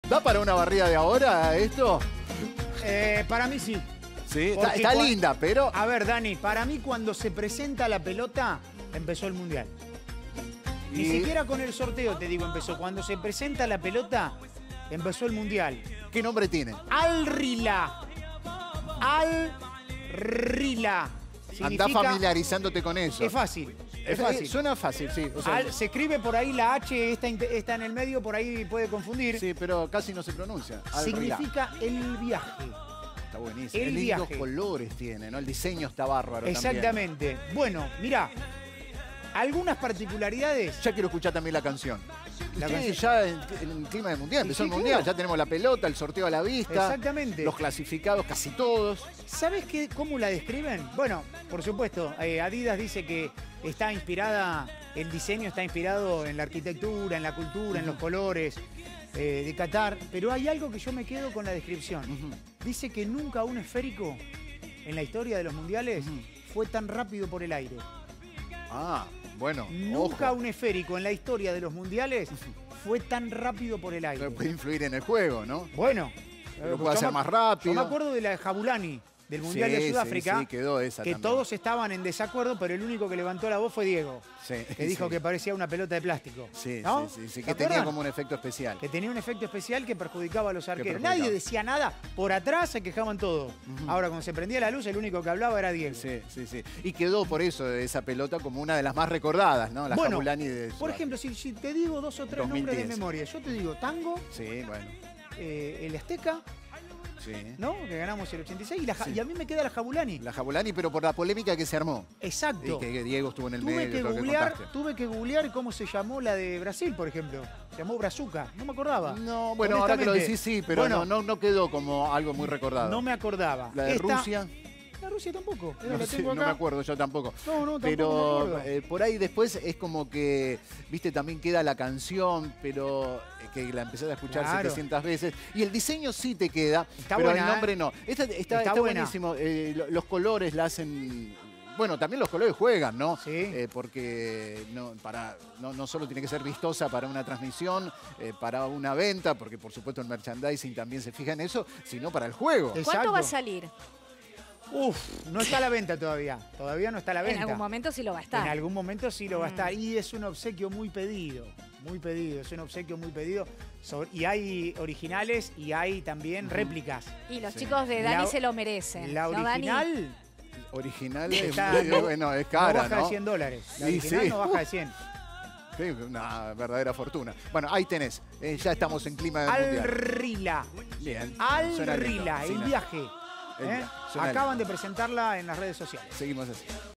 Esta es "Al Rihla", la pelota del Mundial de Qatar | 9 de Área
Este miércoles será el lanzamiento oficial en la Argentina, en el Obelisco y en el Monumento a la Bandera.